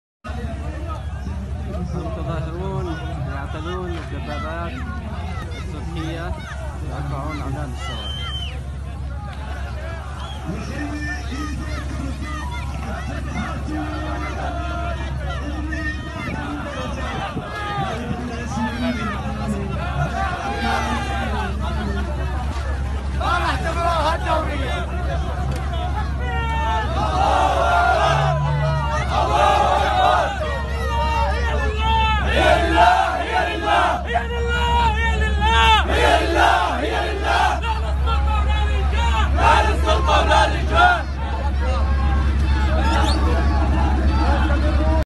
Civilian sit-in continues on the M4 highway
Civilian sit-in continues on the M4 highway, where the sit-in coordination tells us that they continue to sit-in until their demands are met.